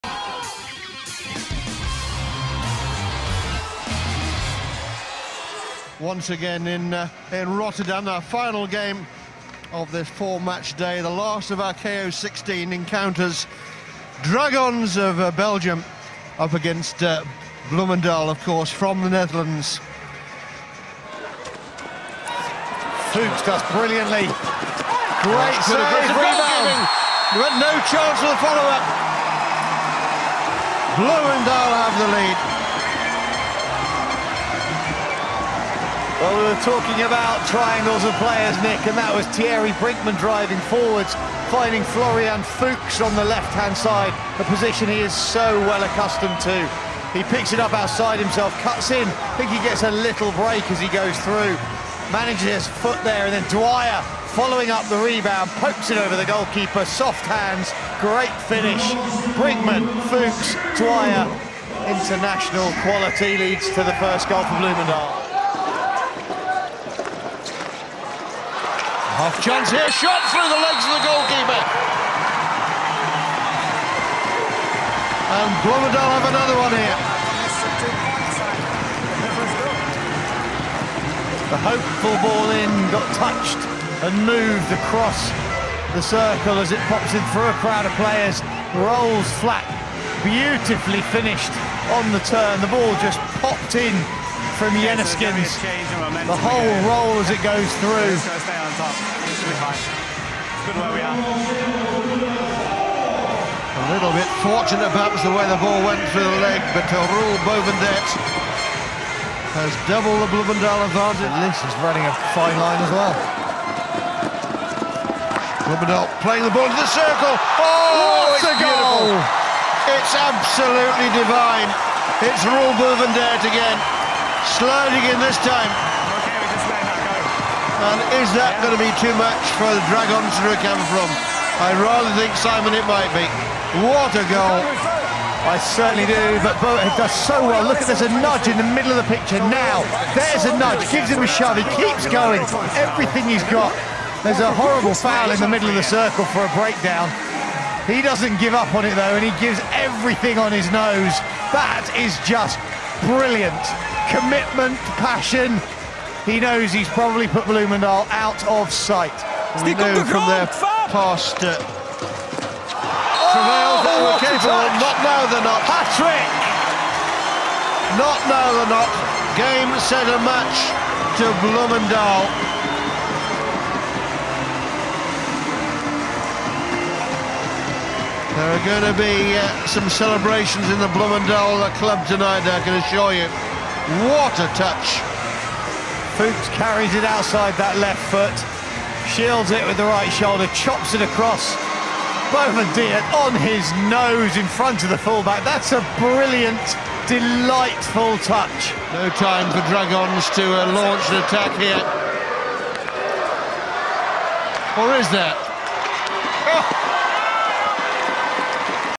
EHL KO16 - Dragons - Bloemendaal (0-8)
Samenvatting van het Euro Hockey League KO16-duel tussen Dragons en Bloemendaal.